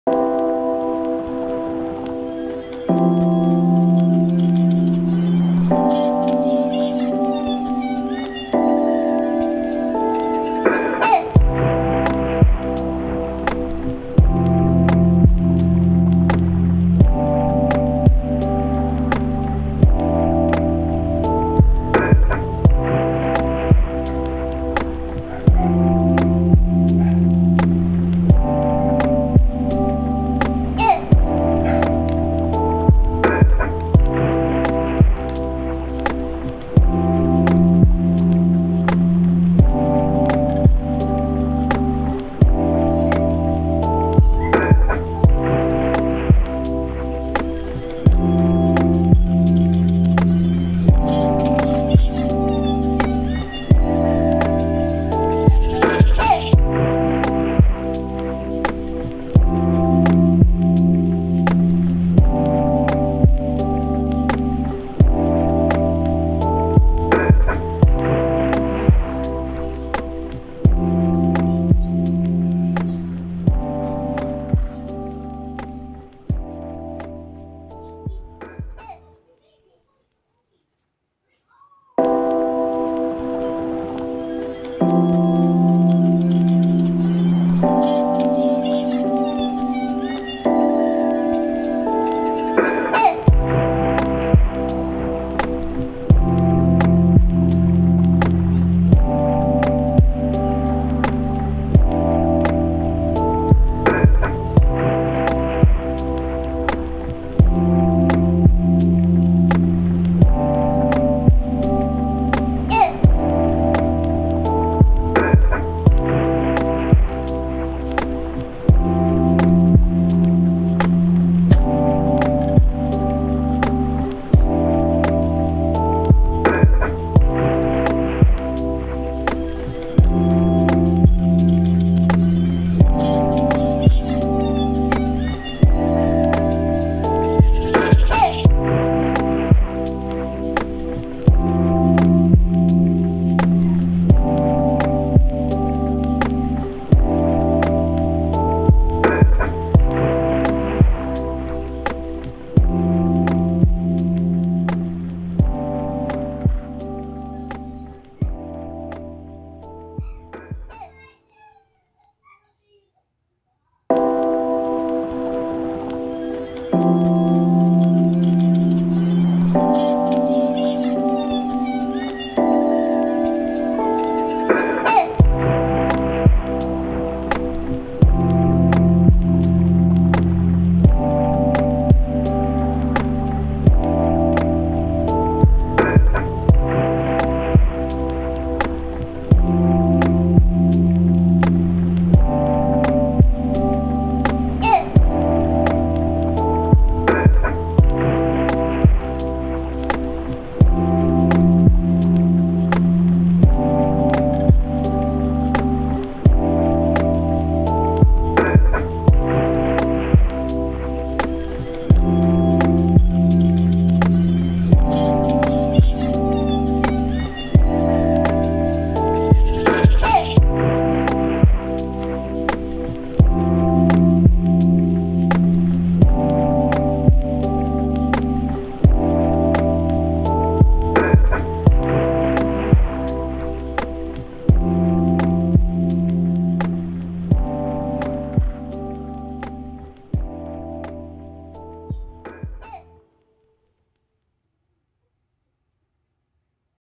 Pentel Vistage Slim Water Brush: A First Impression/ Mini Review | Selah Soul Art
I recently got a Pentel Vistage slim water brush. I researched for good reviews which led me to purchase this particular brush.:) Watch the skin and water drop test to see how this water brush behaves. :)